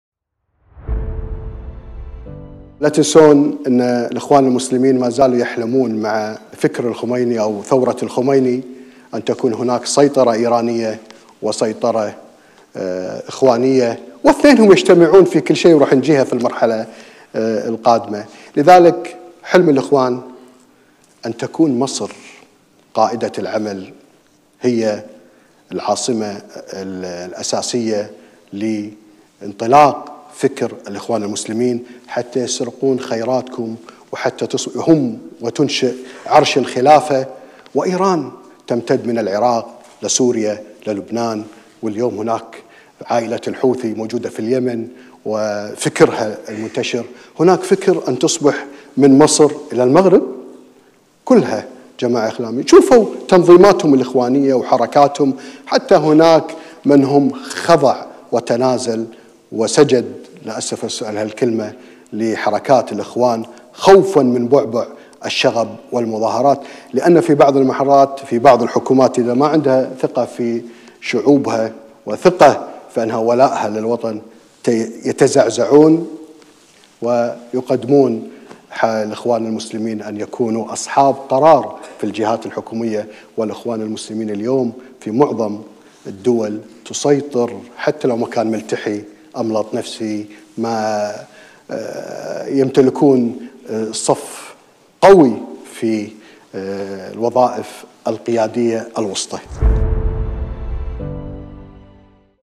إضاءات من محاضرة: صناعة الإرهاب وخطورة الاخوان المسلمين وحزب الله على المنطقة
الإعلامي الكويتي الأستاذ/ محمد أحمد الملا